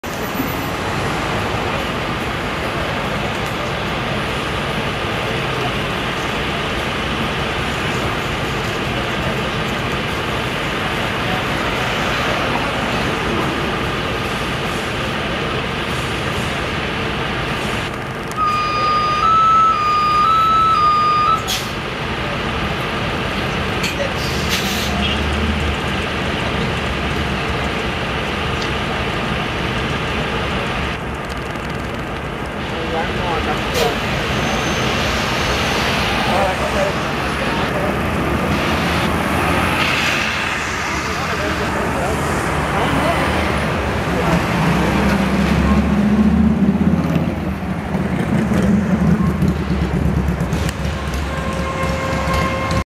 NJT NABI 40-SFW #5881 on the 84 to North Bergen (Outside) in HD
on Bergenline Avenue in West New York headed Northbound.  Too Bad those ROACH COACHES (STINKY JITNEYS) are ruining my Bus Videos again; I'm pissed and those Jersey Jitneys Smell like Piss, JUST PLAIN GET RID OF THEM; the Traffic Jams they cause on Bergenline Avenue are horrendous!!!  Available in High Definition.